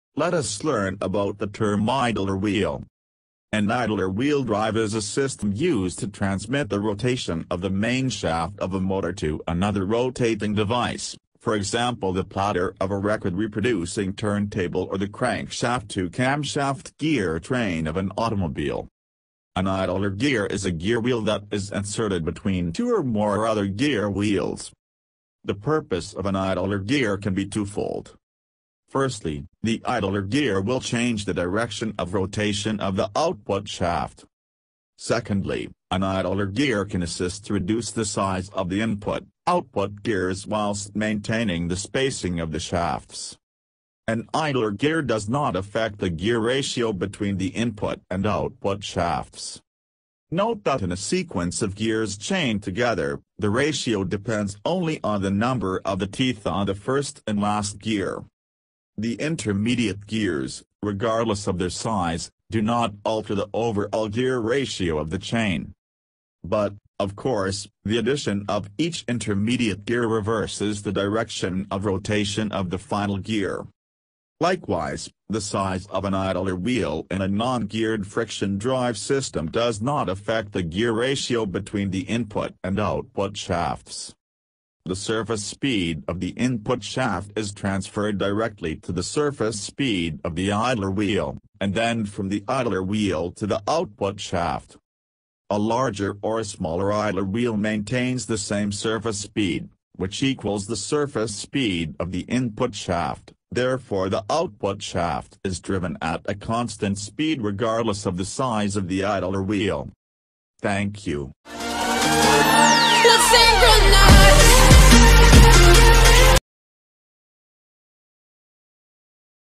Idler Wheel ✔
this video is all about the term Idler-Wheel under mechanical engineering .

let  us  learn  about  the  term   idler-wheel.

An  idler-wheel  drive  is  a  system  used  to  transmit  the  rotation  of the  main  shaft  of  a  motor  to  another  rotating  device,  for  example the  platter  of  a  record-reproducing  turntable  or  the  crankshaft-to-camshaft  gear  train  of  an  automobile.

An  idler  gear  is  a  gear  wheel  that  is  inserted  between  two  or  more  other  gear  wheels.

The  purpose  of  an  idler  gear  can  be  two-fold.

 Firstly,  the  idler  gear  will  change  the  direction  of  rotation  of the  output  shaft.

 Secondly,  an  idler  gear  can  assist  to  reduce  the  size  of  the input/output  gears  whilst  maintaining  the  spacing  of  the  shafts.

An  idler  gear  does  not  affect  the  gear  ratio  between  the  input  and output  shafts.

 Note  that  in  a  sequence  of  gears  chained  together,  the  ratio depends  only  on  the  number  of  teeth  on  the  first  and  last  gear.

The  intermediate  gears,  regardless  of  their  size,  do  not  alter  the overall  gear  ratio  of  the  chain.

But,  of  course,  the  addition  of  each  intermediate  gear  reverses  the direction  of  rotation  of  the  final  gear.

Likewise,  the  size  of  an  idler  wheel  in  a  non-geared  friction  drive system  does  not  affect  the  gear  ratio  between  the  input  and  output shafts.

 The  surface  speed  of  the  input  shaft  is  transferred  directly  to  the  surface  speed  of  the  idler  wheel,  and  then  from  the  idler  wheel  to  the  output  shaft.

A  larger  or  smaller  idler  wheel  maintains  the  same  surface  speed (which  equals  the  surface  speed  of  the  input  shaft),  therefore  the output  shaft  is  driven  at  a  constant  speed  regardless  of  the  size of  the  idler  wheel.